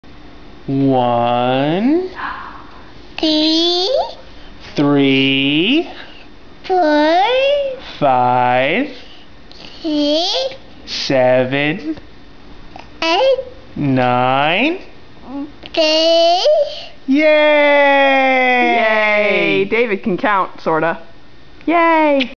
David Alboum can count - sort of.
David's not doing too badly with his counting.  He demonstrates his knowledge here of the even numbers.